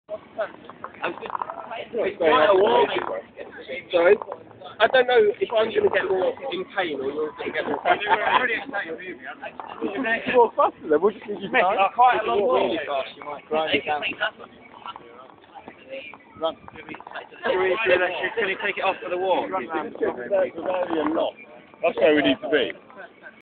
Ball and chain
Ball and chain on a stag do.  Proper cast iron too!